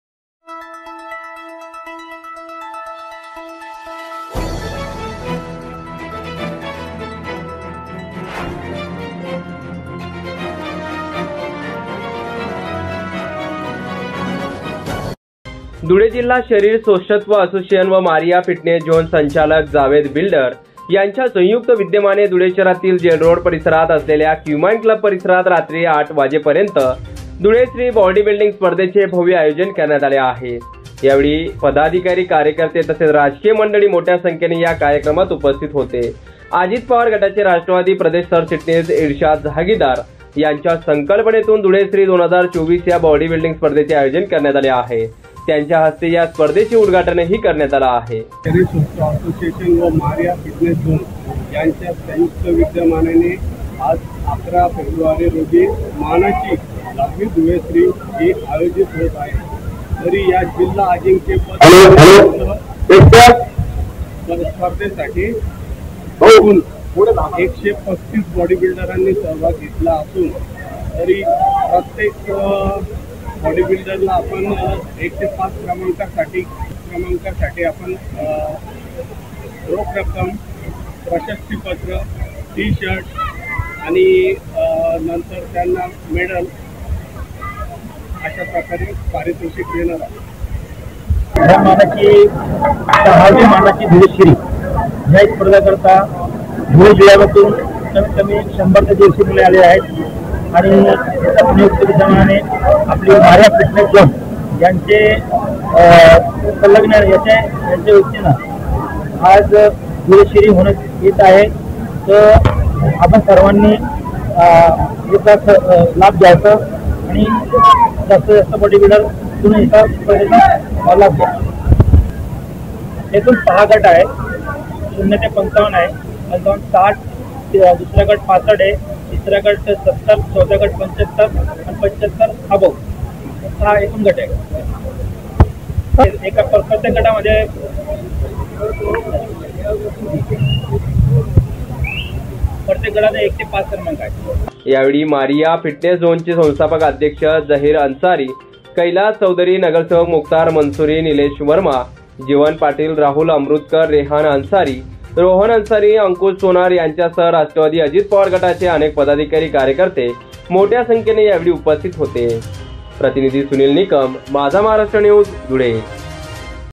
धुळ्यात रंगली सौष्ठव स्पर्धा...धुळ्यात श्री बॉडी बिल्डिंग 2024 स्पर्धेचे आयोजन....
धुळे जिल्हा शरीर सौष्ठव असोसिएशन व मारीया फिटनेस जोन संचालक जावेद बिल्डर यांच्या संयुक्त विद्यपाने धुळे शहरातील जेल रोड परिसरात असलेल्या क्यूमाईन क्लब परिसरात रात्री आठ वाजेपर्यंत धुळे श्री बॉडी बिल्डिंग स्पर्धेचे भव्य आयोजन करण्यात आले होते. यावेळी आणि पदाधिकारी कार्यकर्ते तसेच राजकीय मंडळी मोठ्या संख्येने या कार्यक्रमात उपस्थित होते..